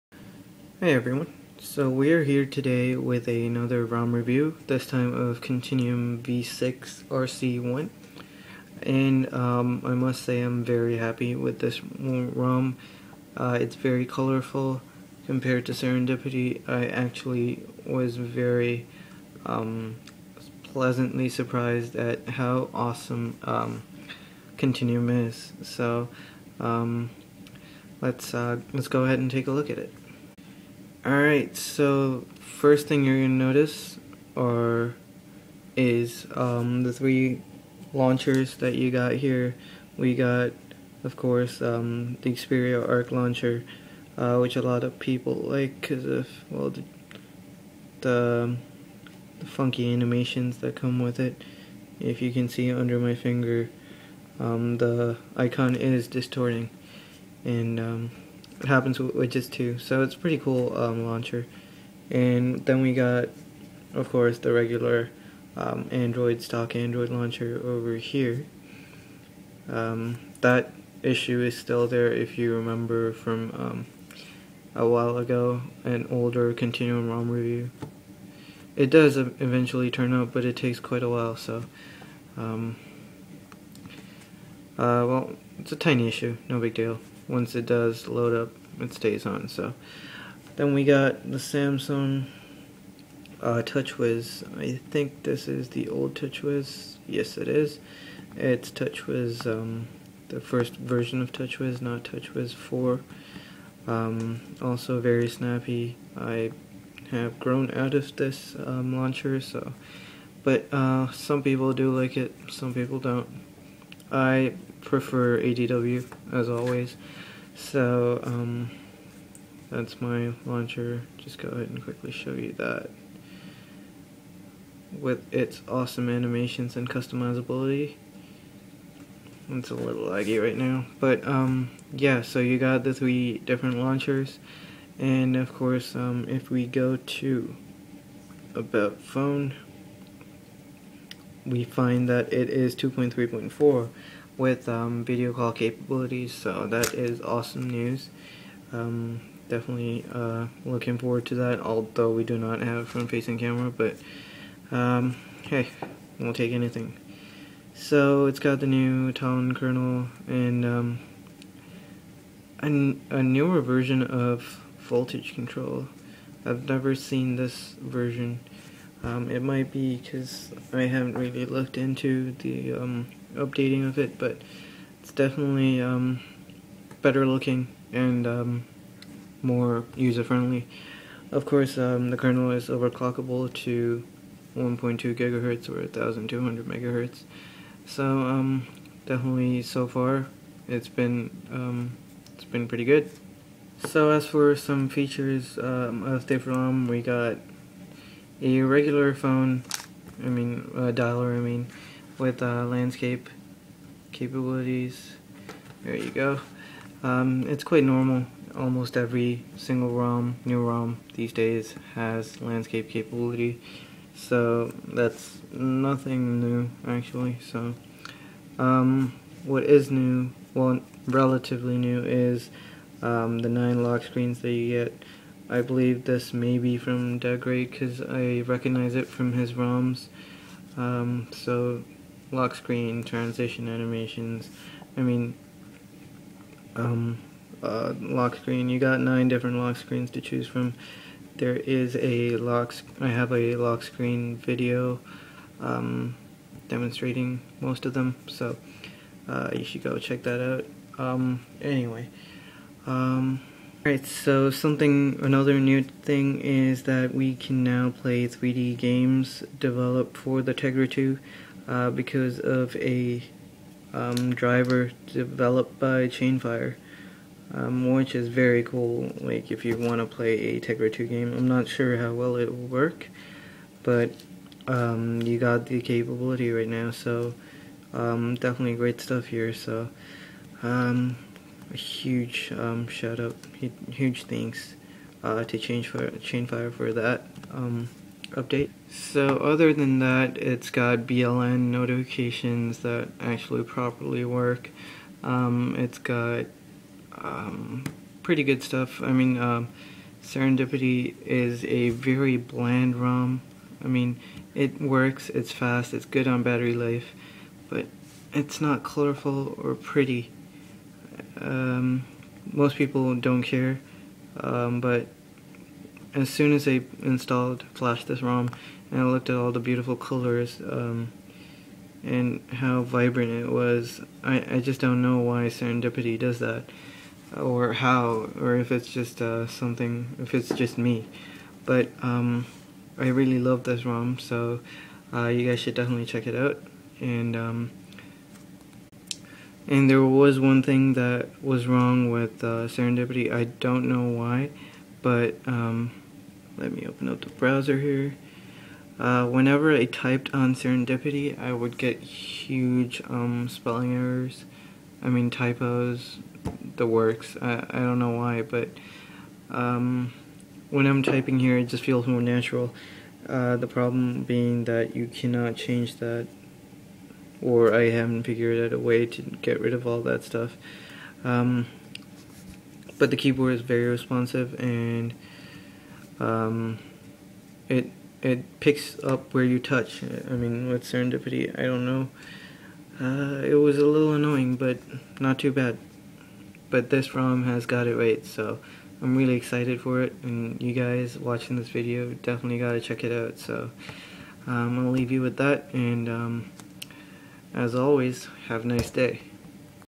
ROM review of Continuum v6 RC1 (2.3.4)
Such a refreshingly good Rom with great functionality.

Read around the post if you want to change your bootloarders back to ATT bootloaders

I am not responsible for any damage that you may do to your phone.